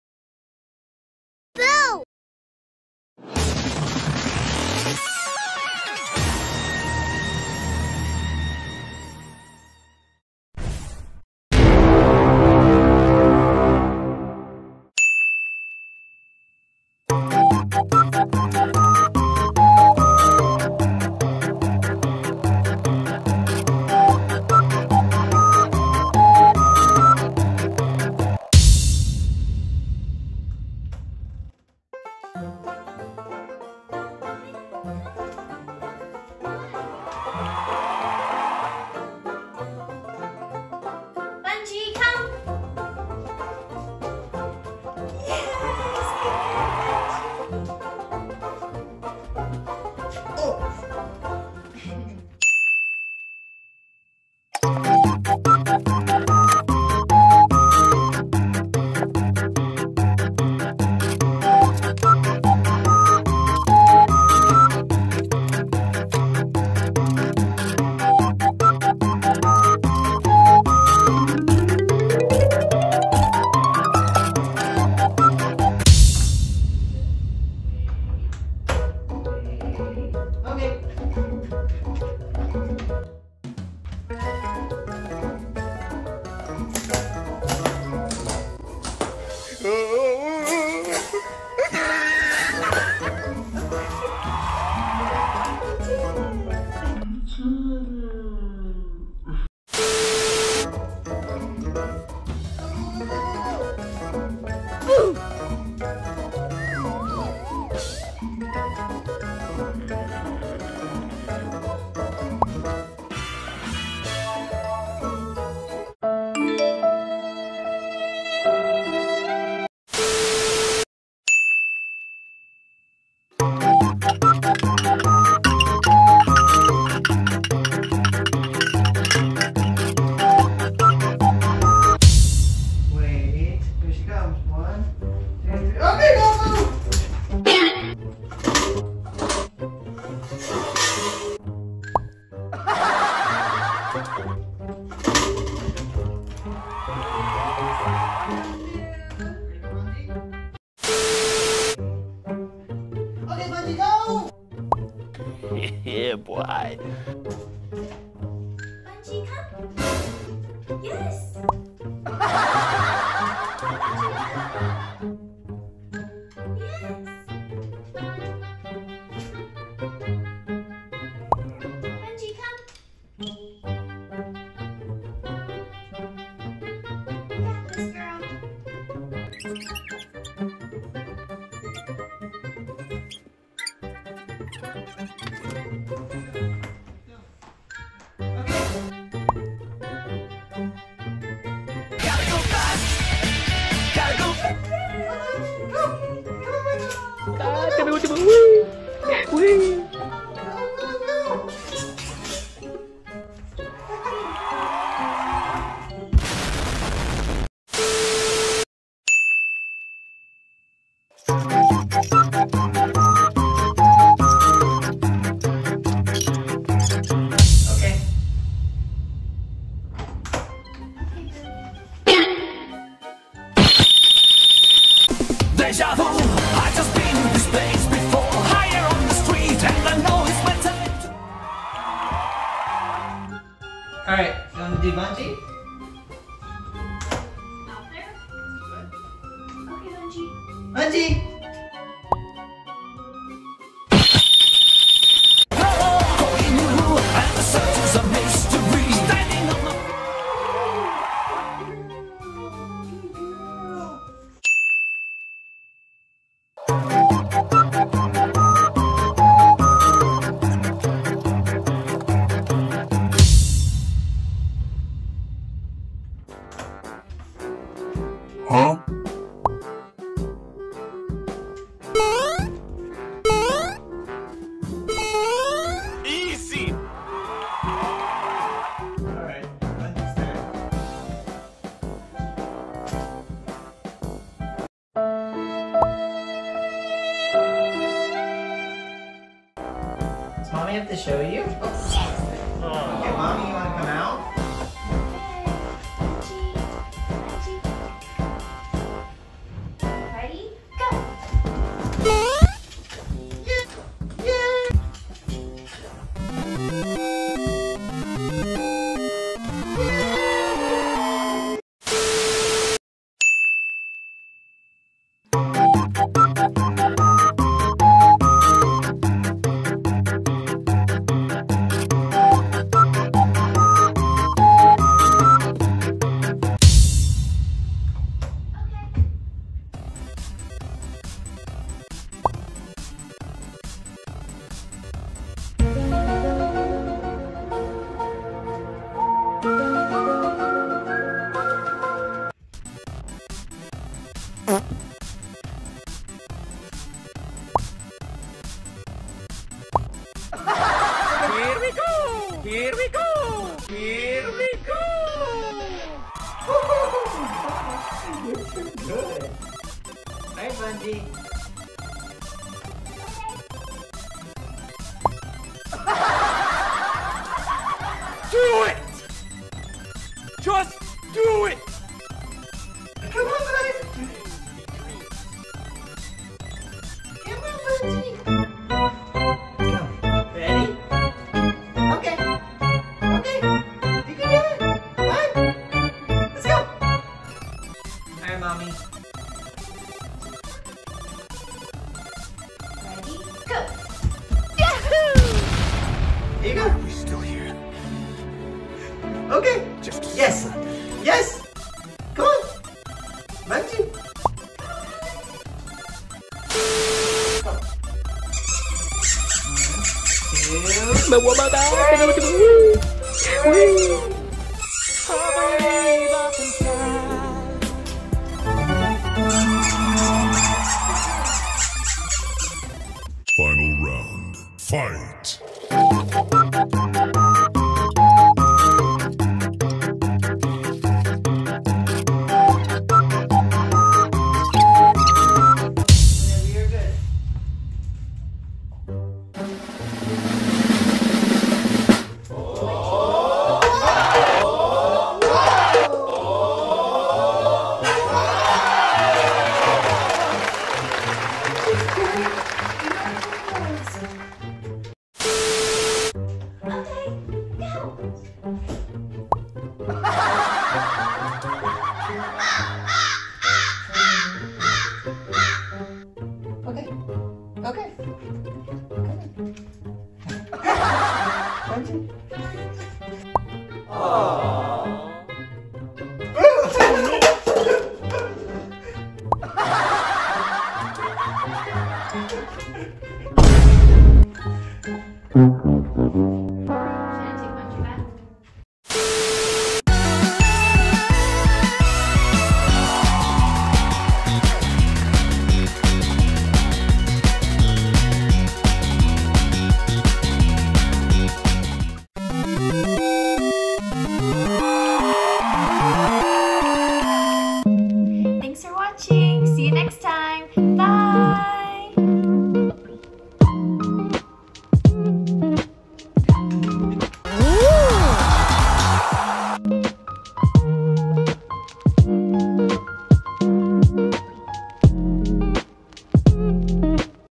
Dog Obstacle Challenge: SAMOYED vs. COCKER SPANIEL
In this Dog Obstacle Challenge, we're comparing two of the most popular dog breeds: the Samoyed vs. the Cocker Spaniel.

Both of these breeds are known for their intelligence and their athleticism. In this challenge, we'll see which of these breeds is better at overcoming obstacles. Will the Samoyed be able to navigate the obstacles quickly and efficiently, or will the Cocker Spaniel be able to get through them with ease? Let's find out!